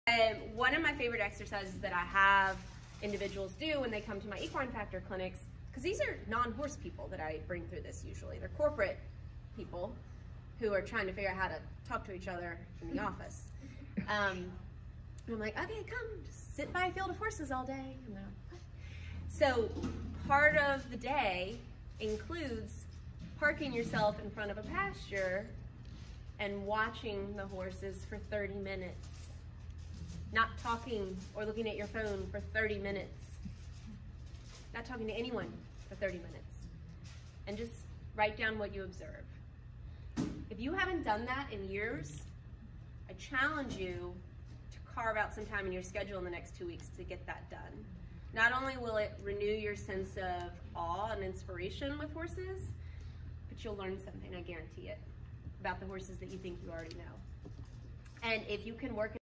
Equine Factor: The Power of Observation
A glimpse of my talk to the TRAV association, discussing the power of observation when 'Speaking with Horses' through my Equine Coaching program.